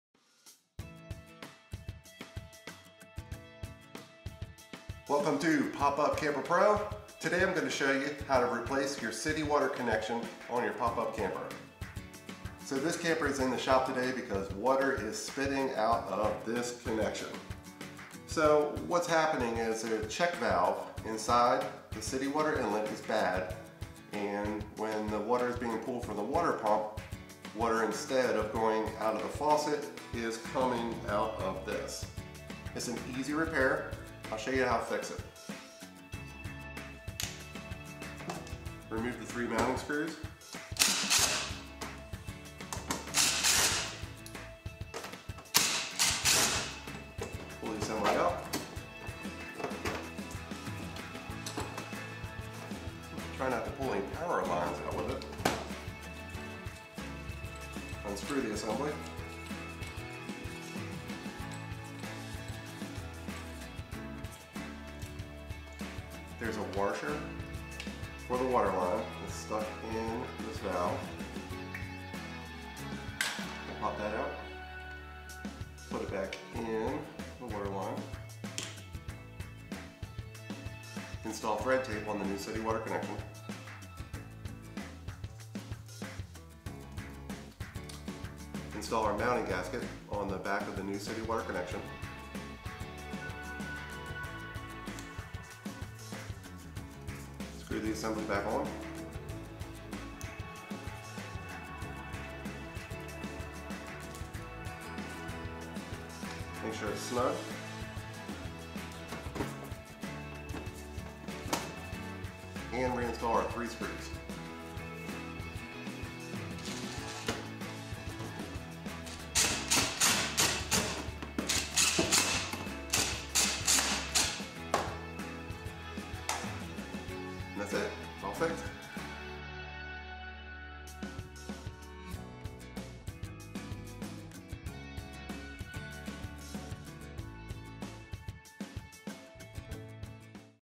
How to replace a city water inlet on a camper
If water is coming out of the city water inlet on your camper, this tutorial will explain how to fix the issue.

Official pop up camper pro merchandise is available! please to see what new art and products Derek Smith has created.